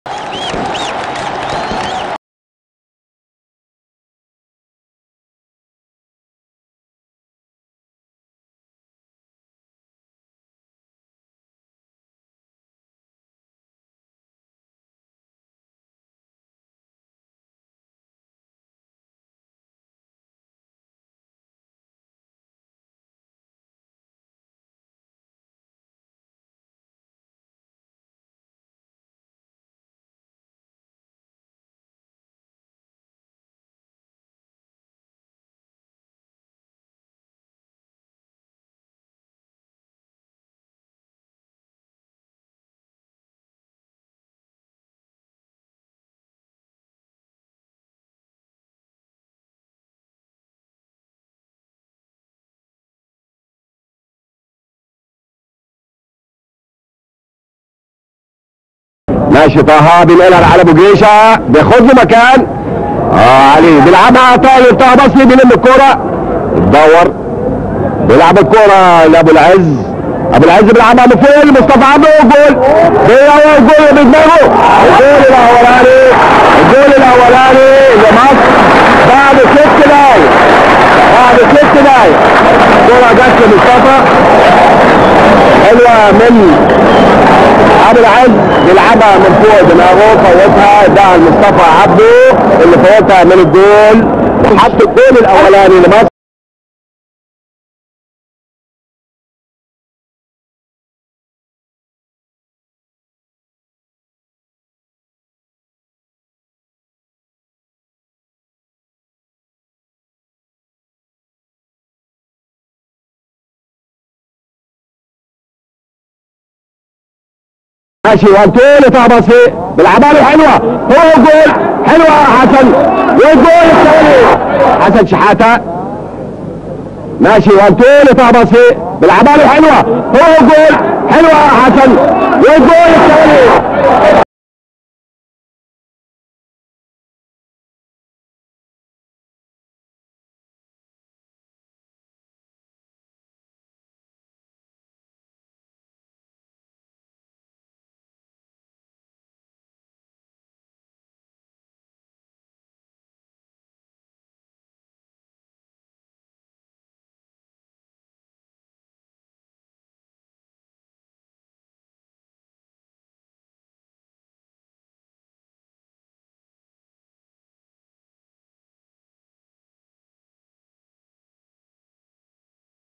مصر 4 - 0 الكونغو - كأس الأمم الأفريقية مصر 1974
Date: 11/3/1974

3rd Place Playoff - African Nations Cup

Egypt 4 - 0 Congo Brazzaville

Scorers: Moustafa Abdou 8'  - Hassan Shehata 18', 80' - Ali Abougreisha 62'

Egyptian Line Up:

Ikrami
Raafat Mekki
Mohamed aboul Ezz
Hassan Darwish
Hany Moustafa
Taha Basry
Farouk Gaafar
Hassan Shehata
Moustafa Abdou (Hassan El-Shazly)
Ali Abougreisha
Mahmoud El-Khateeb (Sayed Abdel Razek)